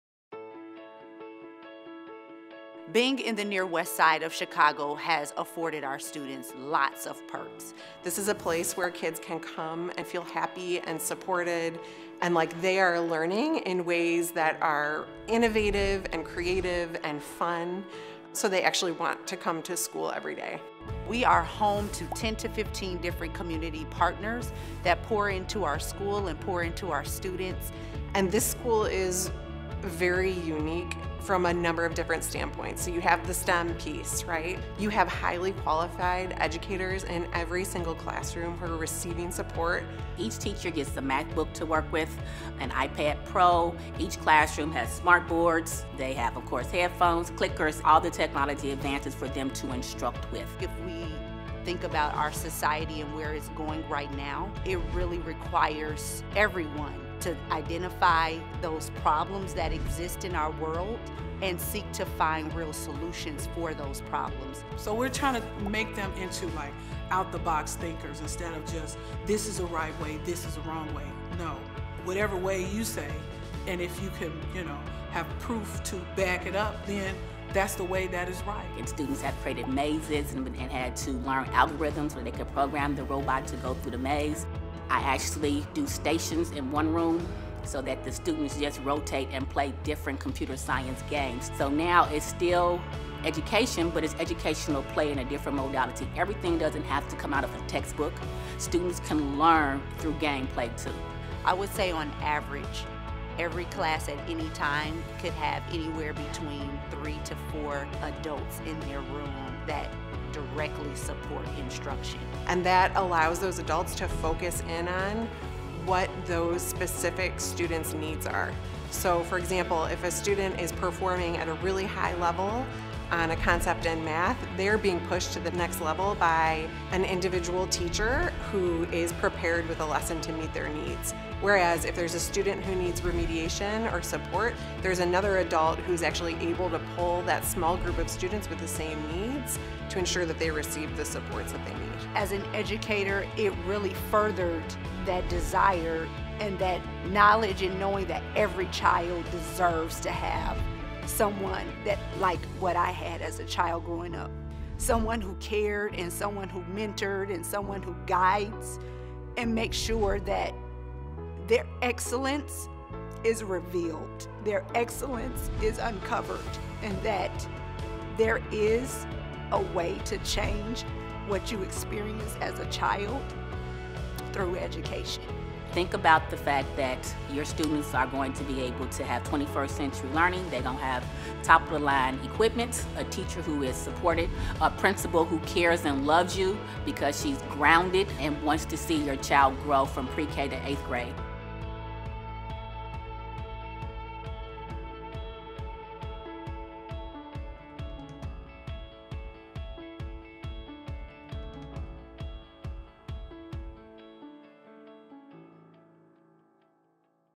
Inside Look into William H Brown STEM Magnet School
Choose a school where you have a principal who cares, a rigorous STEM curriculum, and a place where we believe in cultivating the gifts and talents of your child! Located on the Near West side of Chicago, IL